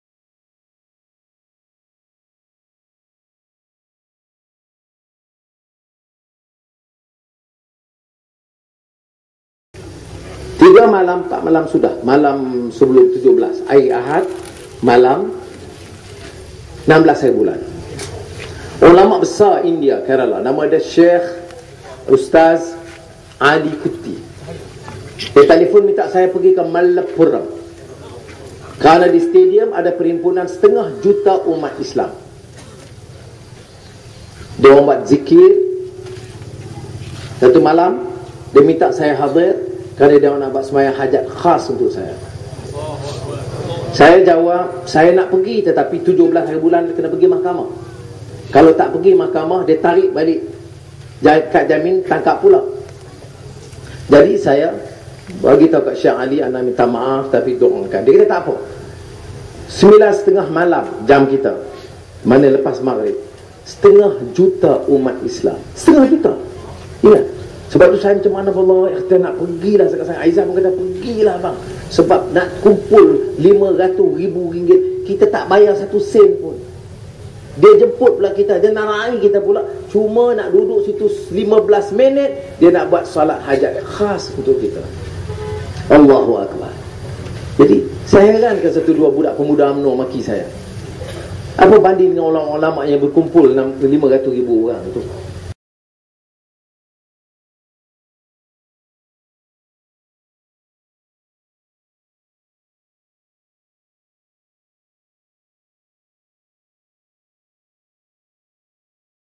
Ulamak India bersama 500 Ribu Umat Islam doakan AlJuburi
Apa yang menarik perhatian aku, RajaDrama ini memulakan ceramahnya dengan mengungkap kisah yang kononnya dia difitnah, kemudian menyusun selepas itu ulama' India bersama 500 ribu umat Islam (selepas itu Aljuburi kata 500 Ribu ulama' pulak) kononnya akan membuat solat hajat khas untuk AlJuburi. Terfikir pula aku, kenapa di India? Apa kena mengena dengan Malaysia terutama sekali apa sangkut paut sumbangan AlJuburi untuk India sehingga dibuat solat hajat khas untuk AlJuburi?